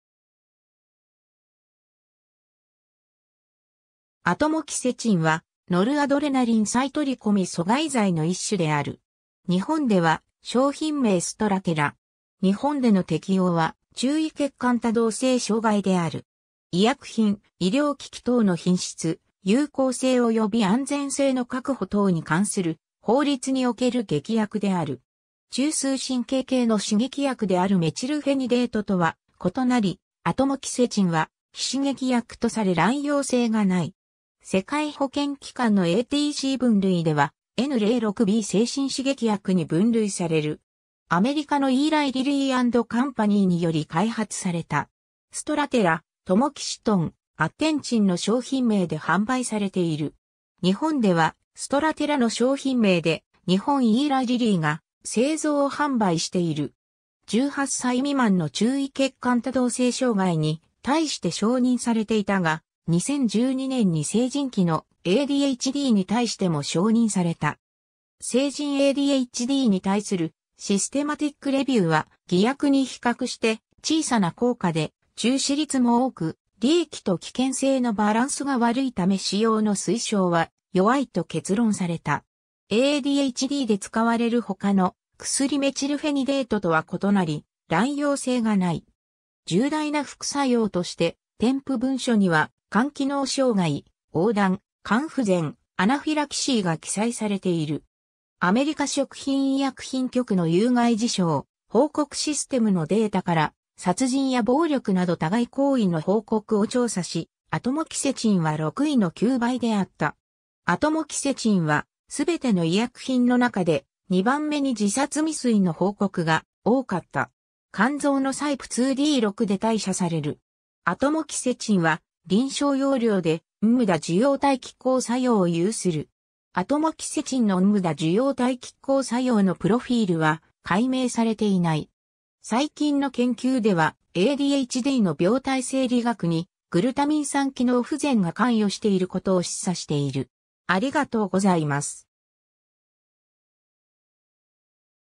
アトモキセチン
セロトニン・ノルアドレナリン再取り込み阻害薬
アトモキセチン（英語: Atomoxetine）はノルアドレナリン再取り込み阻害剤の一種である。
日本では商品名ストラテラ。
日本での適応は注意欠陥多動性障害(ADHD)である。
医薬品、医療機器等の品質、有効性及び安全性の確保等に関する法律における劇薬である。
 中枢神経系の刺激薬であるメチルフェニデートとは異なり、アトモキセチンは非刺激薬とされ乱用性がない。
世界保健機関 (WHO) のATC分類ではN06B-精神刺激薬に分類される。
 アメリカのイーライリリー・アンド・カンパニーにより開発された。
Strattera、Tomoxetin、Attentinの商品名で販売されている。
 日本ではストラテラ (Strattera)の商品名で、日本イーライリリーが製造販売している。
18歳未満の注意欠陥多動性障害(ADHD)に対して承認されていたが、2012年に成人期のADHDに対しても承認された。
 成人ADHDに対するシステマティックレビューは、偽薬に比較して小さな効果で中止率も多く、利益と危険性のバランスが悪いため使用の推奨は弱いと結論された。
 ADHDで使われる他の薬メチルフェニデートとは異なり、乱用性がない。
 重大な副作用として、添付文書には肝機能障害、黄疸、肝不全、アナフィラキシーが記載されている。
 アメリカ食品医薬品局(FDA)の有害事象報告システム(AERS)のデータから殺人や暴力など他害行為の報告を調査し、アトモキセチンは6位の9倍であった。
 アトモキセチンは全ての医薬品の中で、2番目に自殺未遂の報告が多かった。
 肝臓のCYP2D6で代謝される。
 アトモキセチンは臨床用量でNMDA受容体拮抗作用を有する。
 アトモキセチンのNMDA受容体拮抗作用のプロフィールは解明されていない。
最近の研究ではADHDの病態生理学にグルタミン酸機能不全が関与していることを示唆している。